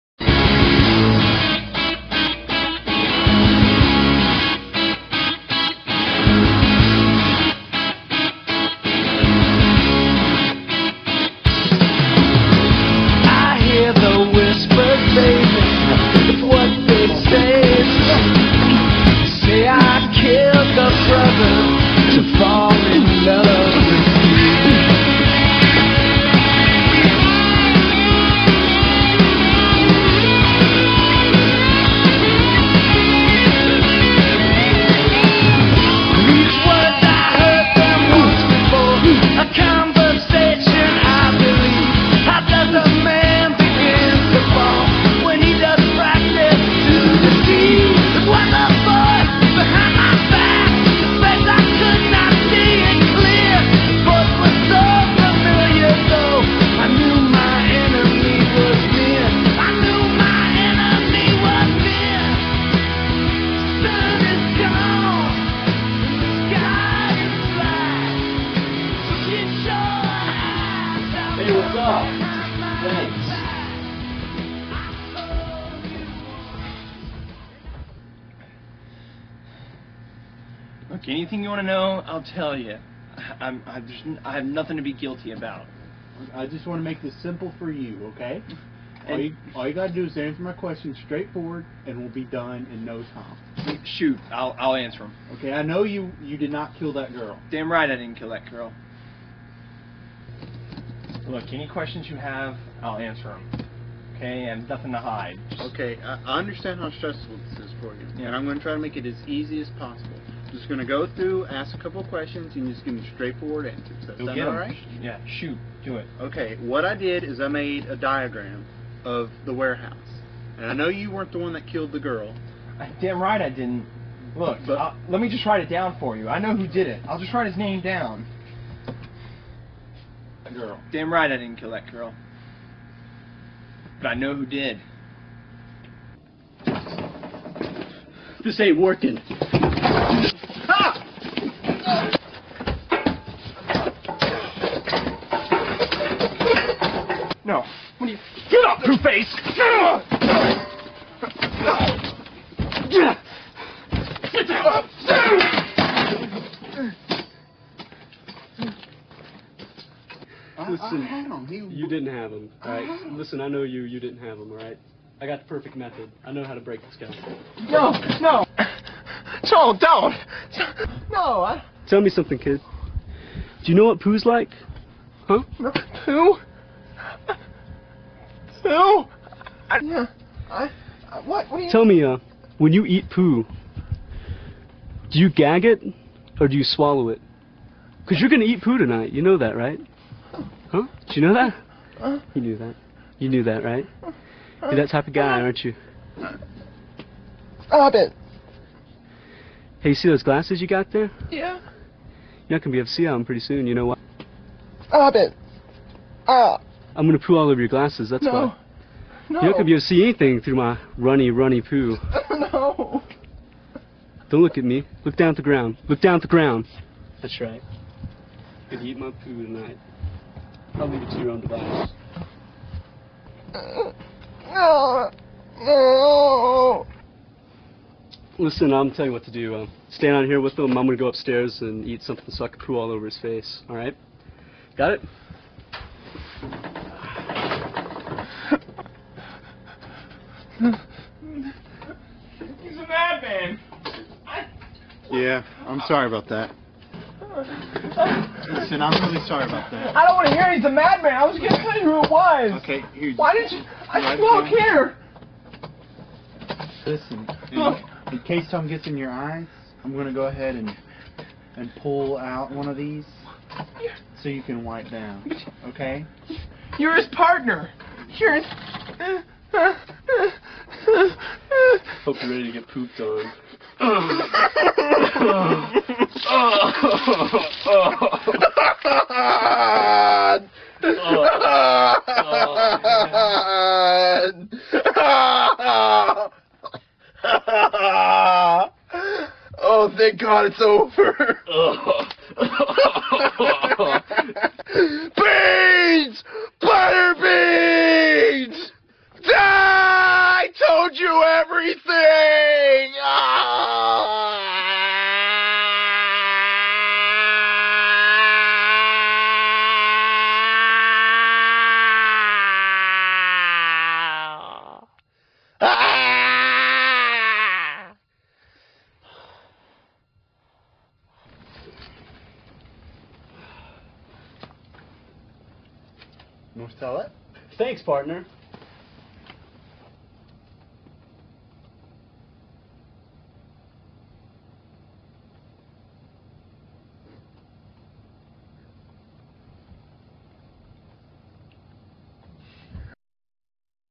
Joe Straight & Kid FURIOIUS in Balls-Out Detectives "interrogation"
Joe Straight & Kid FURIOIUS in Balls-Out Detectives "interrogation" warning includes chili torture